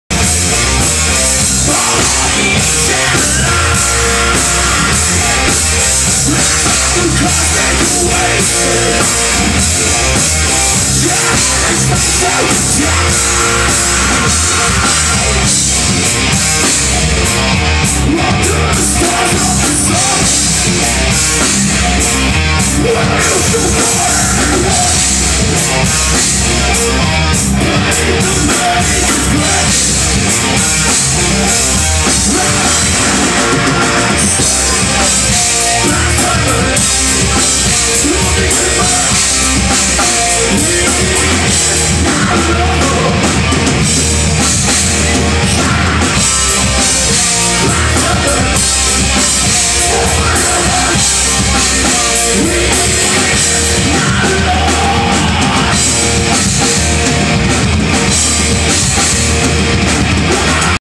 The Black Swamp - Sydney Frankies 30 Nov 2014
The Black Swamp - live in Sydney Frankies Pizza by the Slice. 30 Nov 2014. Kentcore metal night! anyone know song name please comment thanks.